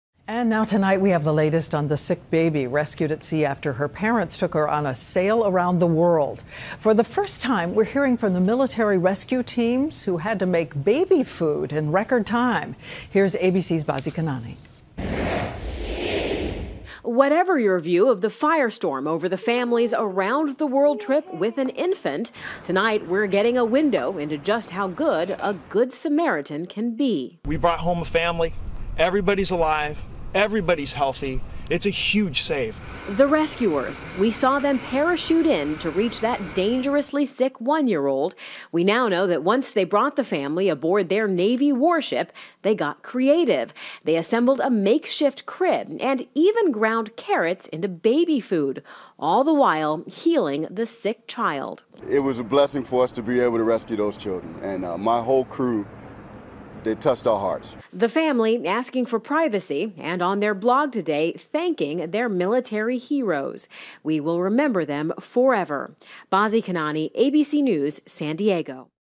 Military Rescue Team Describes Saving Family at Sea With a Sick Bab
National Guard medics describe their heartfelt mission using parachutes; parents express gratitude.